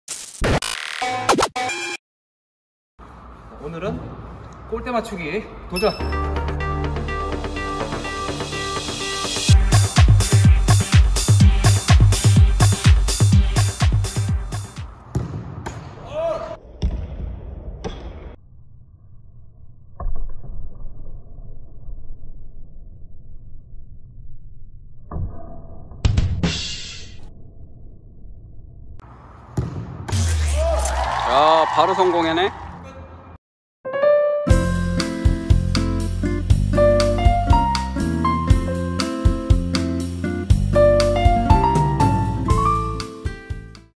골대 맞추기 도전!⚽️
골대 맞추기 도전해봤습니다~

과연 몇 번 만에 맞출지..? ㅋㅋㅋ